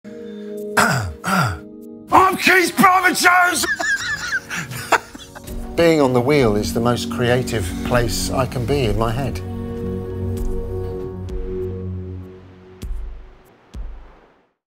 “Being on the wheel is the most creative place I can be in my head.” – Keith
On leaving school, as well as enjoying a role singing in a punk band, Keith pursued his pottery dreams by placing an ad in the ‘Ceramic Review’. The ad read, “Young, enthusiastic 18-year-old seeks apprenticeship in a Pottery” and it quickly found him a job at a pottery studio located on the other side of London. Despite the lengthy commute, ten hour shifts and demanding bosses, Keith remained committed to his craft and eventually he became an accomplished production ‘thrower’. So when the studio relocated to Scotland some years later Keith had gained enough knowledge and confidence to start up his own studio. Although he worried about keeping a new business afloat, his natural affinity for making pots ensured the orders poured in straight away. Now living much nearer to his workplace than previously, Keith would still rise incredibly early each morning to make the thousands of items commissioned by a growing list of prestigious clients.

“Being on the wheel is the most creative place I can be in my head.”GROUNDED